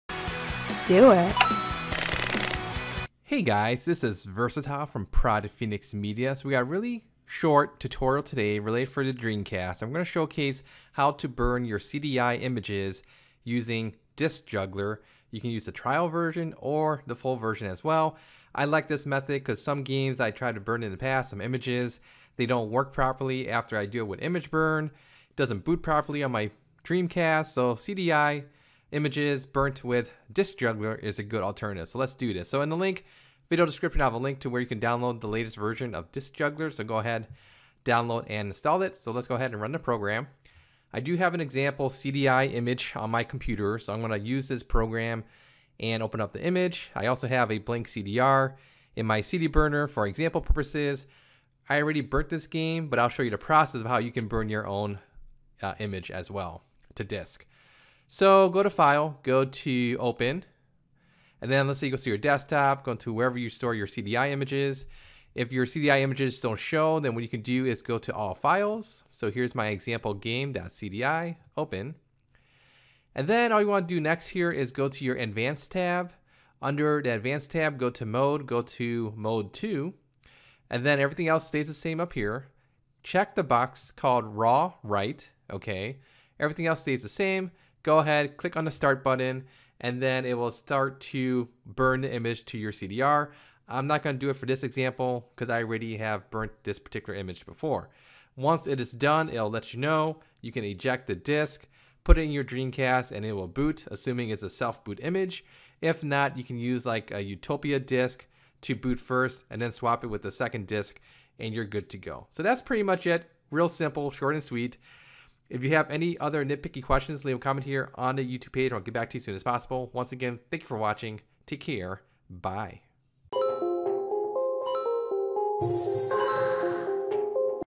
Burn CDI Images Using DiscJuggler 6.0 for Dreamcast (2019)
Today's tutorial is a short refresher on how to use DiscJuggler 6.0 to burn .CDI images for your compatible Dreamcast.

Some game images don't work using IMGBurn, so if you ever get into that scenario, DiscJuggler 6.0 may do the trick. You got nothing to lose and worst case scenario? Get a GDEMU!

Needs a VPN? We recommend Private Internet Access (PIA)

MoNeYINPHX PC Specs